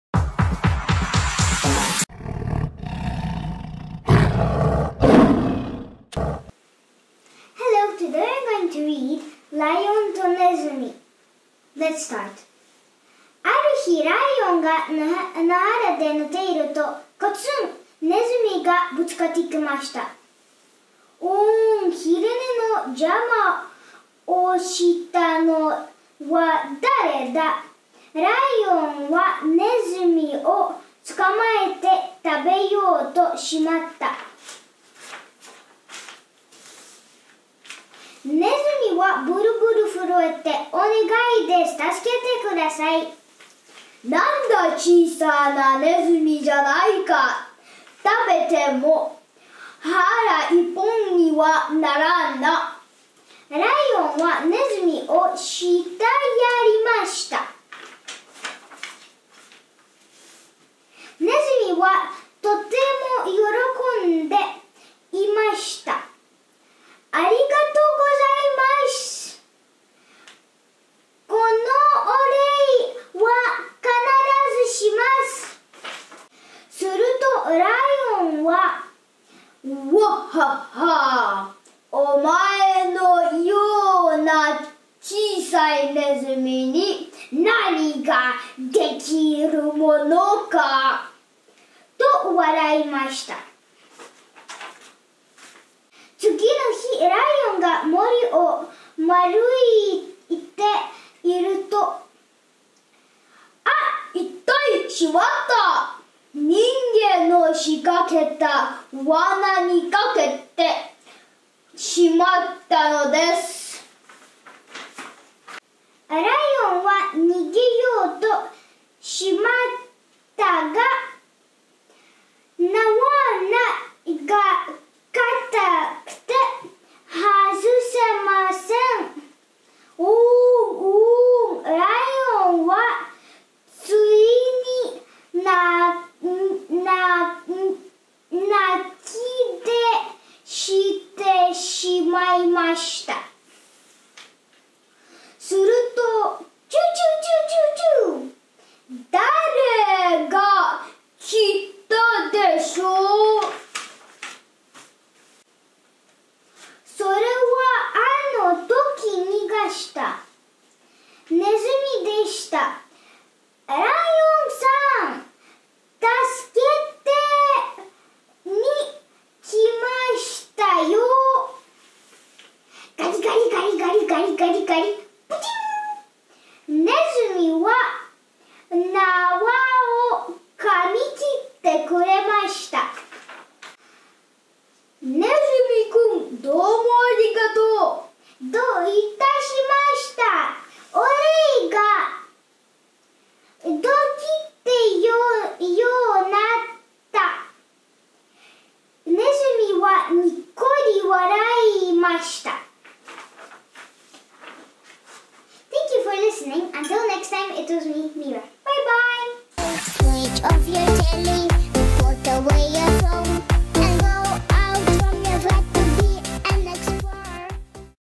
Read with Mira in Japanese - ライオンとネズミ (The lion and the mouse)
Read with Mira in Japanese!
Learn Japanese by reading books.
The story about the Lion and the Mouse (ライオンとネズミ)
with the text to follow.
Let's start!